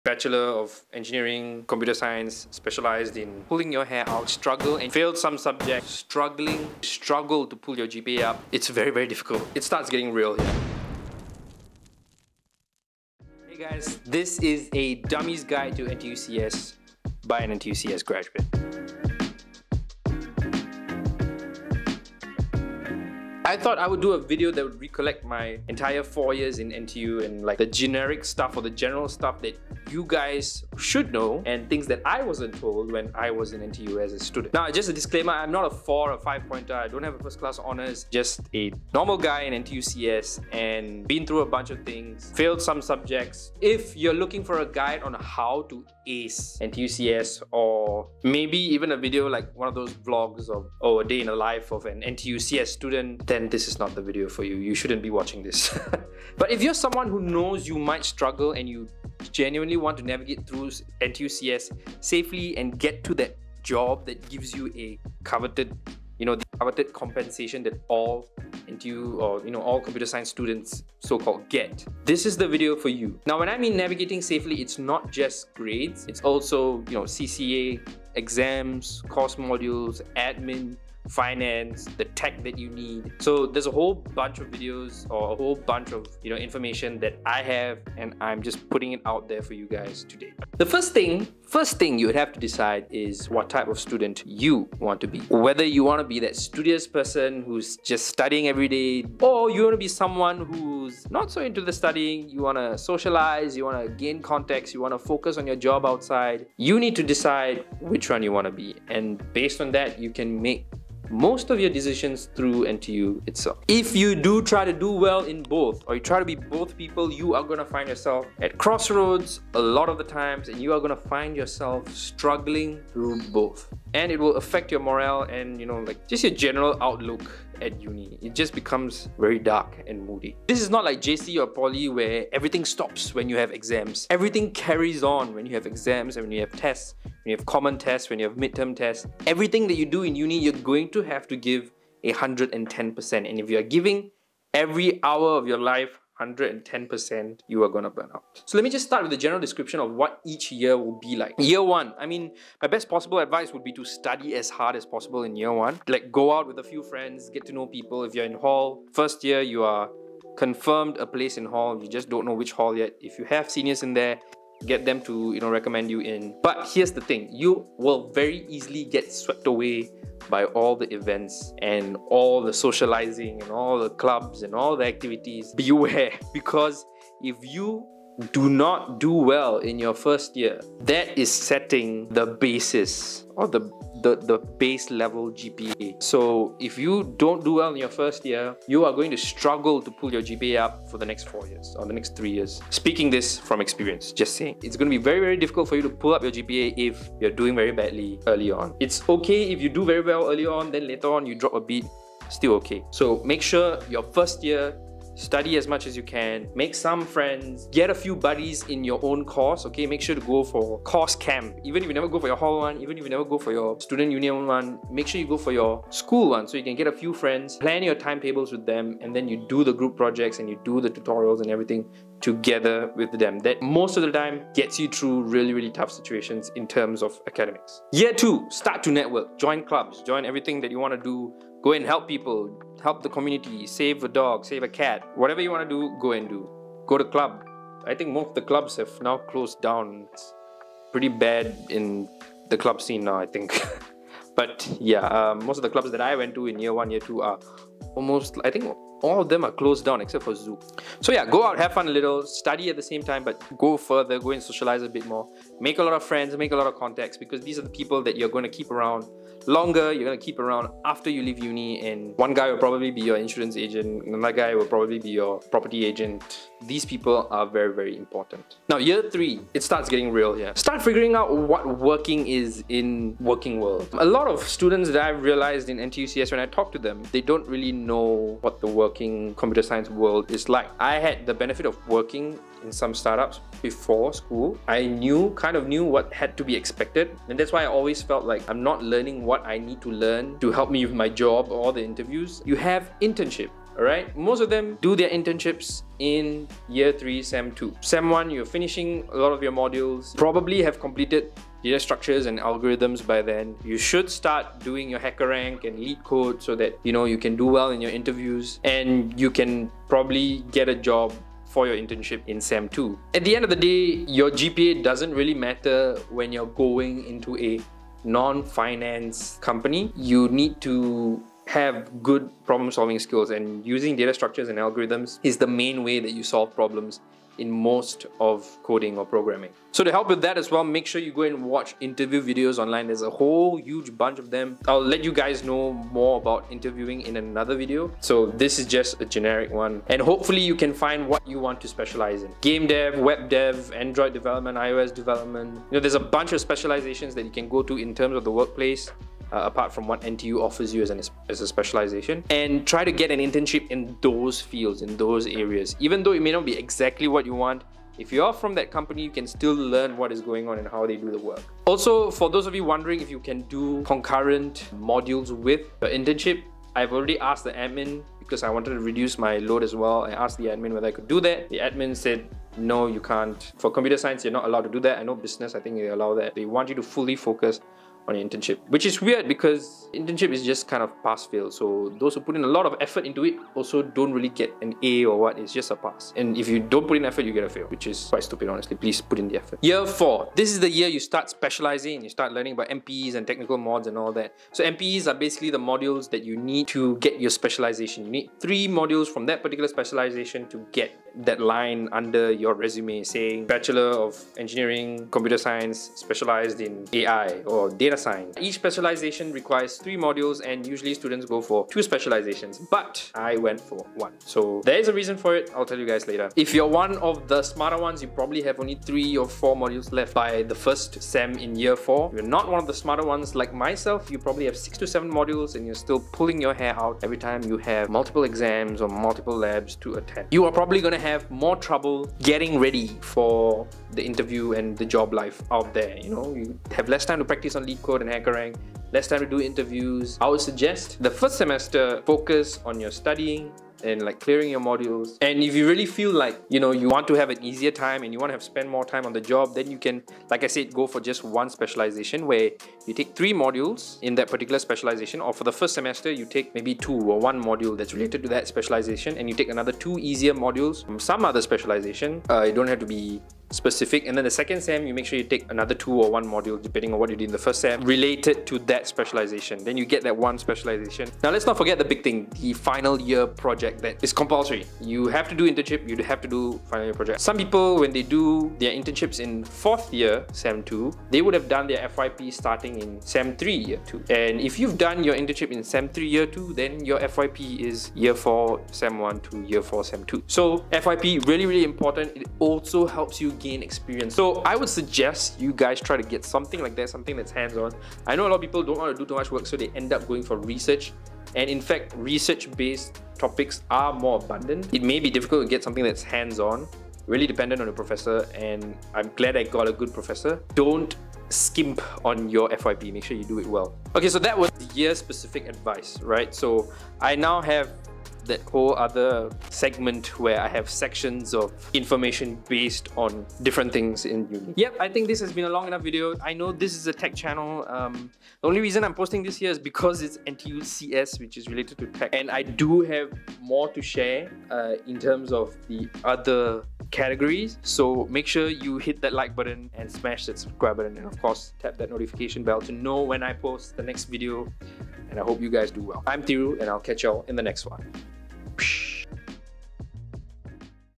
How to survive NTU Computer Science - Year Based Guide (2022)
I've graduated. Now it's your turn! If you're struggling as an NTU CS student, join the club lol.
Everything you need to know about NTU.

Would you rather read it?

Timeline:
00:00 - Introduction
01:30 - The First Question
02:28 - Year 1
03:46 - Year 2
04:26 - Year 3
06:28 - Year 4
08:33 - Outro

Shot On: Canon M50
Audio: Voical Lark X